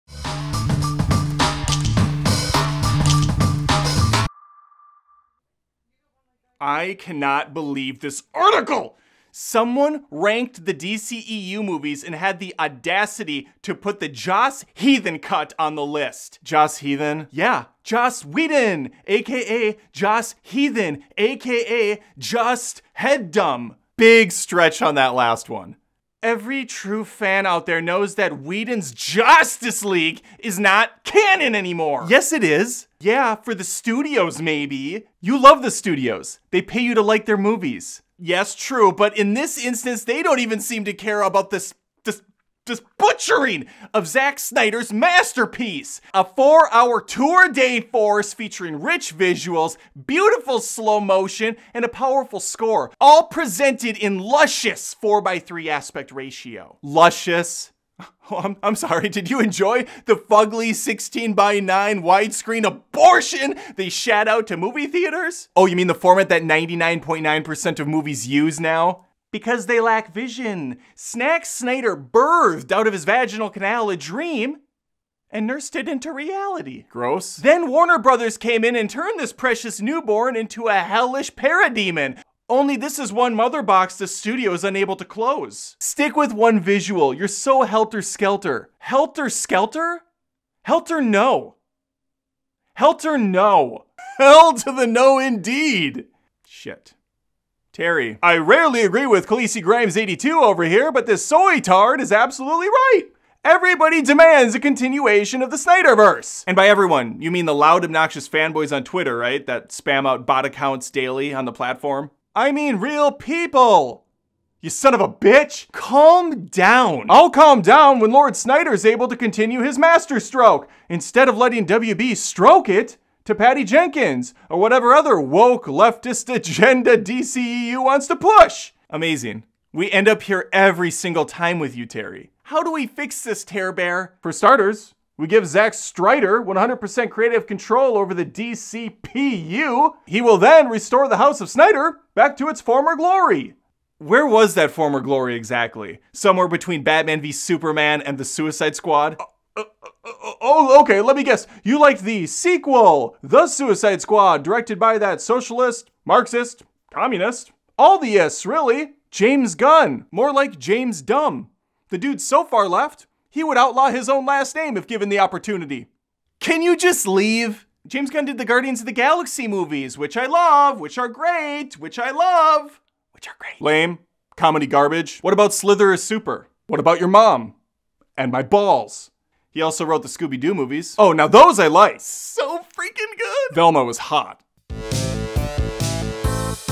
Arguing With The DCEU Snyder Cult - Dumb Debates
The Zack Snyder DCEU fans got what they wanted when HBO Max finally released the 4 hour Snydercut. Now they demand more! Some are on Twitter spamming out Snyder praise and requesting he be reinstated to helm the DCEU. Nothing short of Snyder's return will make these guys happy.